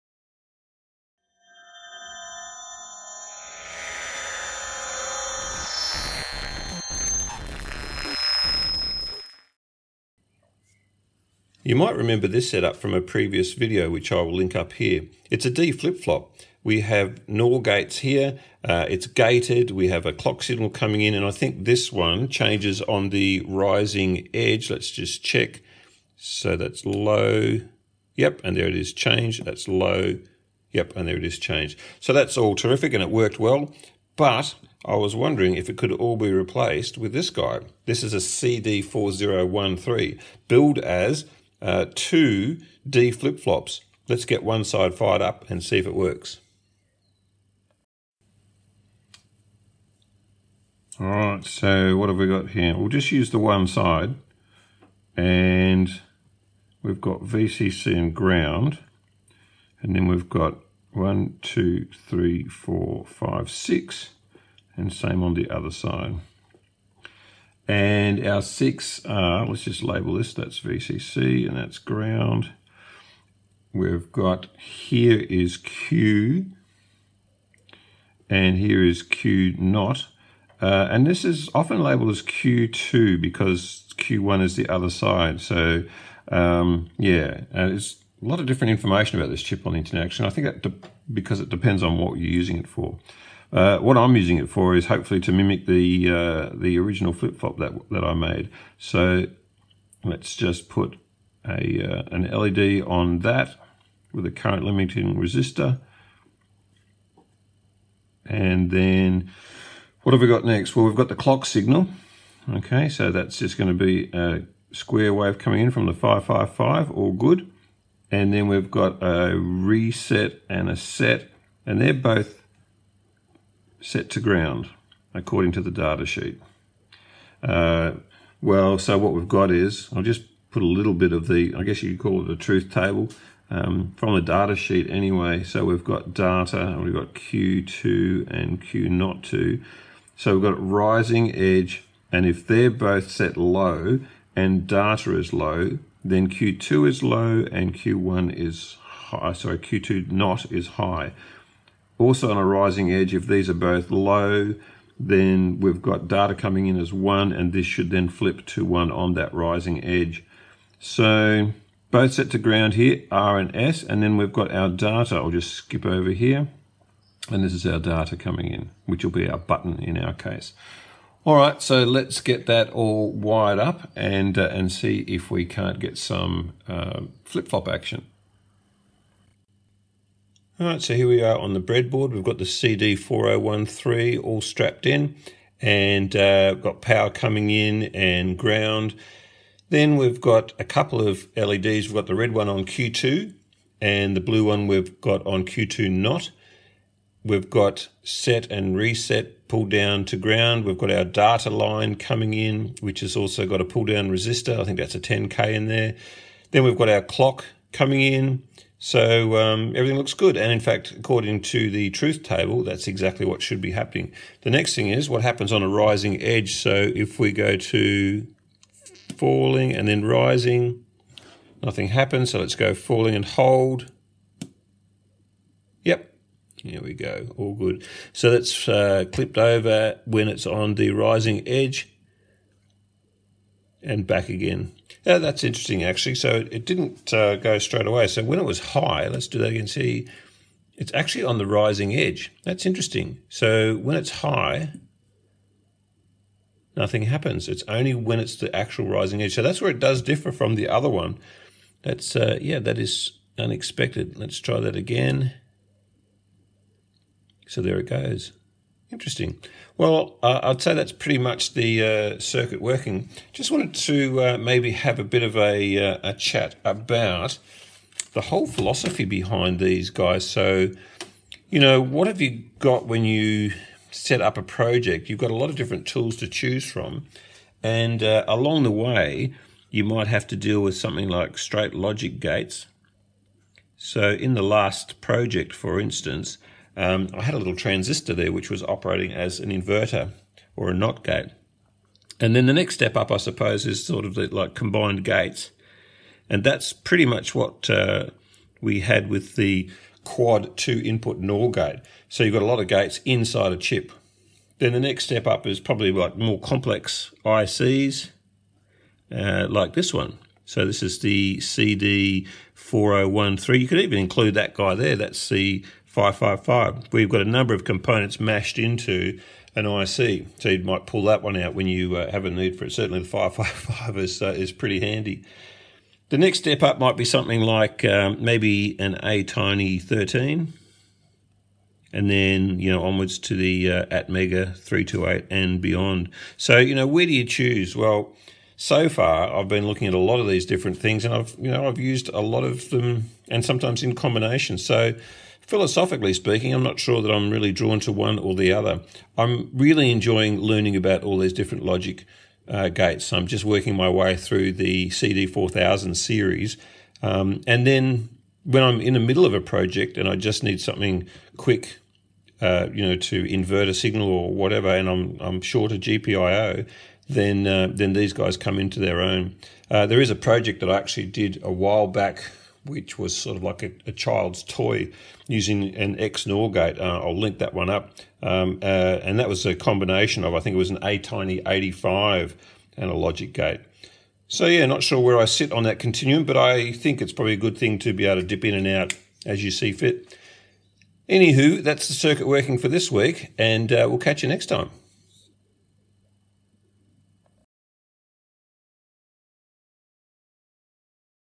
CD4013 Dual D-Type Flip-Flop
Each week I will grab a random electronic component from the vault and build a circuit - this week I look at an "all in one" D Flip Flop, the CD4013